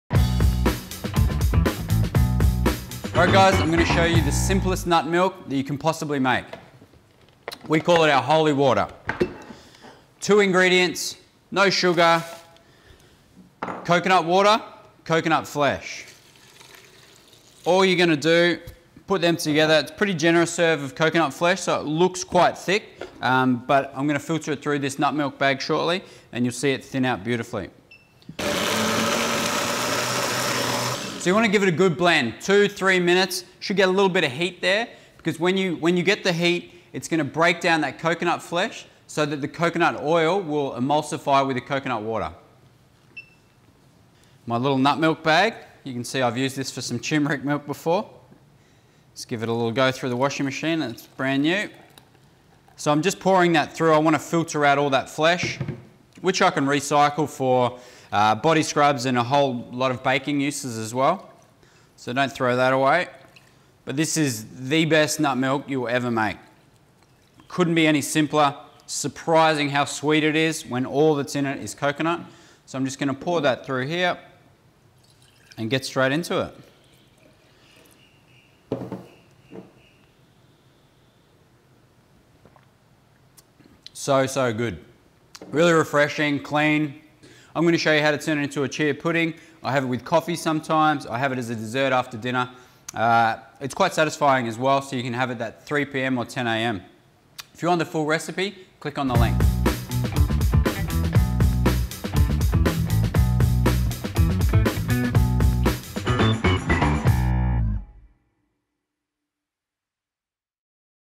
How To Make The World's Greatest 2 Ingredient Nut Milk
All right guys, I'm going to show you the simplest nut milk that you can possibly make. We call it our Holy Water. Two ingredients, no sugar, coconut water, coconut flesh. All you're going to do, put them together, it's a pretty generous serving of coconut flesh, so it looks quite thick, but I'm going to filter it through this nut milk bag shortly, and you'll see it thin out beautifully.

You want to give it a good blend, two-three minutes. Should get a little bit of heat there, because when you get the heat it's going to break down that coconut flesh so that the coconut oil will emulsify with the coconut water. My little nut milk bag, you can see I've used this for some tumeric milk before, just give it a little run through the washing machine and it's brand new. I'm just pouring that through, I want to filter out all that flesh, which I can recycle for body scrubs and a whole lot of baking uses as well, so don't throw that away. But this is the best nut milk you will ever make. Couldn't be any simpler, surprising how sweet it is when all that's in it is coconut.

I'm just going to pour that through here, and get straight into it. So, so good. Really refreshing, clean. I'm going to show you how to turn it into a chia pudding. I have it with coffee sometimes, I have it as a dessert after dinner, it's quite satisfying as well, so you can have it at 3 p.m. or 10 a.m. If you want the full recipe, click on the link.

Coconut water and coconut flesh. You will not find a better nut milk. Easy, sweet, versatile and delicious.

If you want more clean recipes with plenty of flavour then get your browser